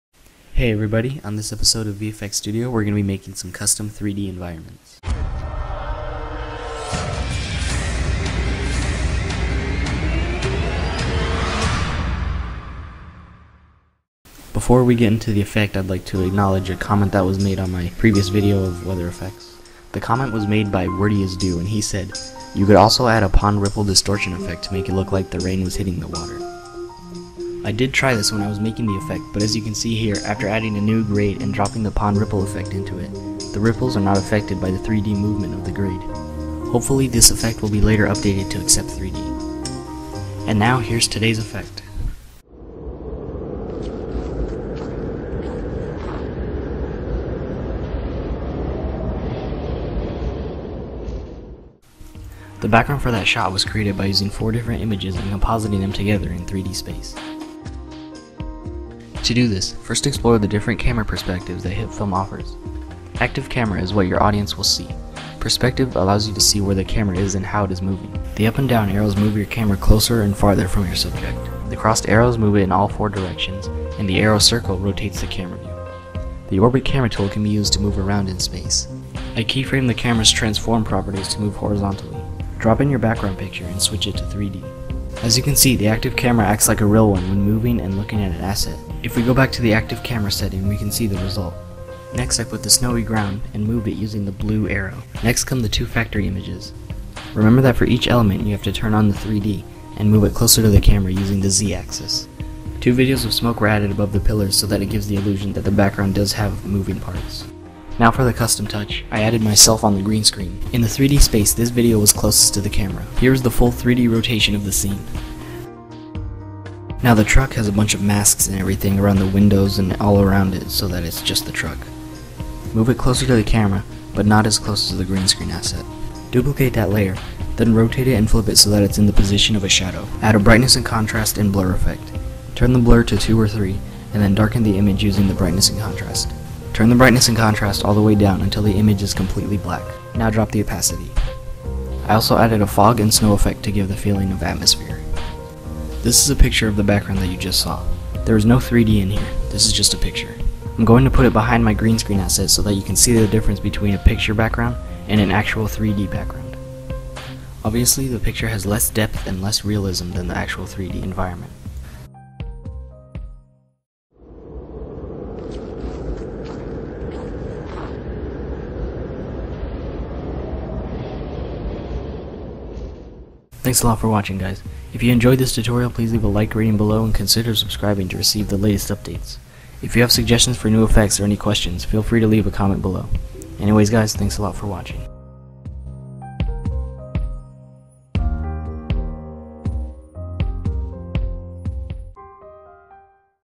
Creating 3D Environments - Set Extensions & Customized Locations
Learn how to create your own sets and studios in HitFilm! Effect starts at 00:43.

Next Episode: Superhero Contest Entries